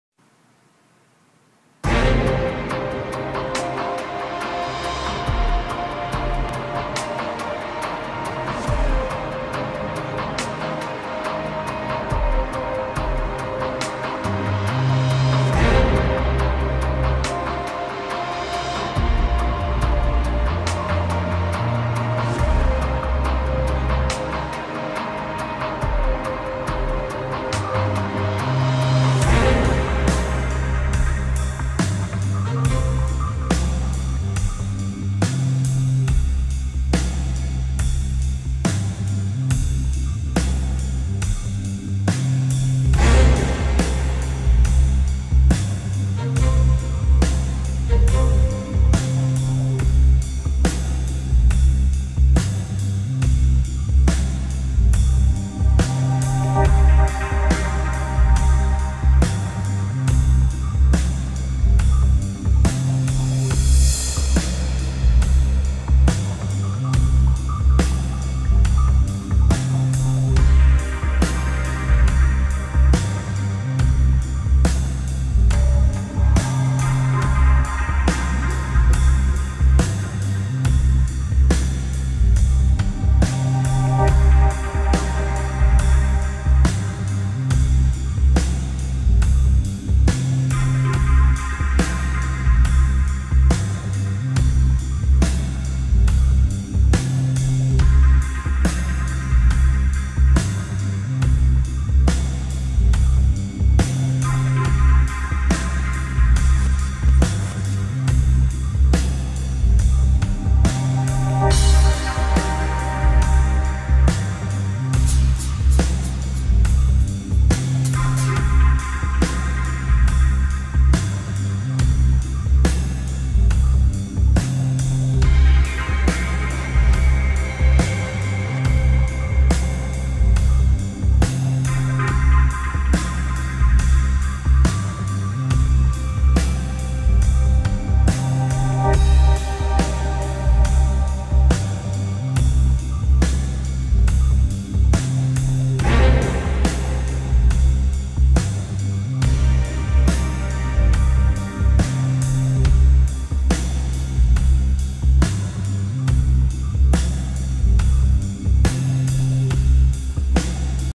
Portrait Oil Painting Liam, Recorded upside down : (
14 hr oil painting, which was recorded upside down :(. Artist Chris Kochan